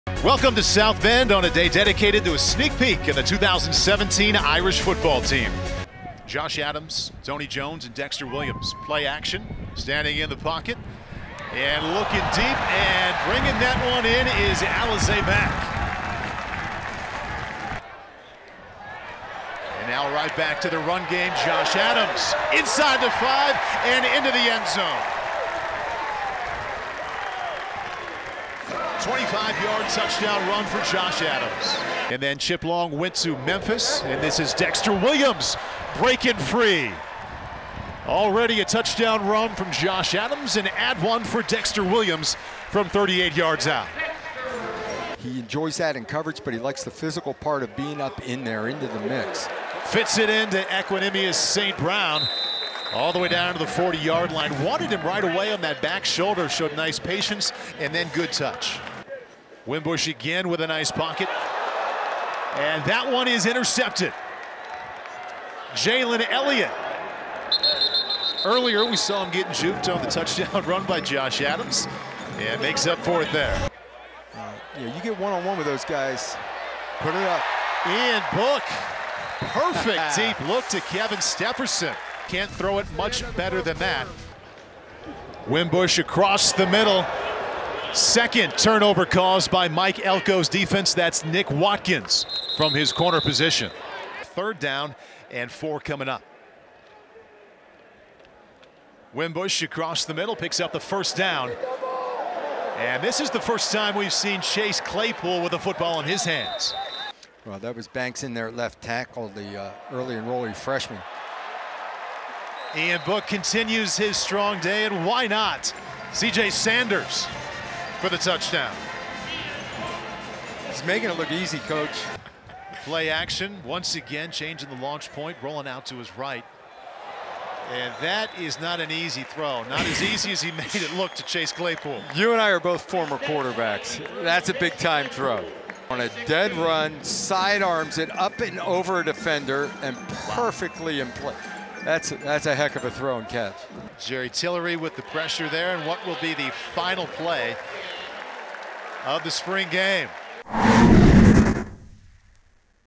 Highlights - 2017 Notre Dame Football Blue-Gold Game
Highlights from the 2017 Notre Dame Football Blue-Gold Game